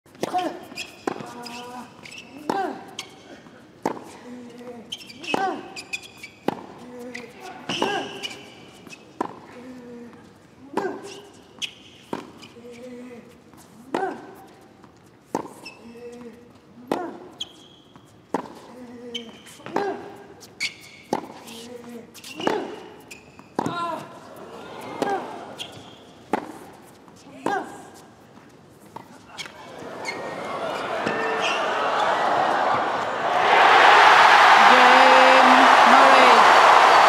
Andy Murray's pure GRIT! 💪
Andy Murray showed his pure grit in this rally against Roberto Bautista Agut in the first round of the Australlian Open 2019.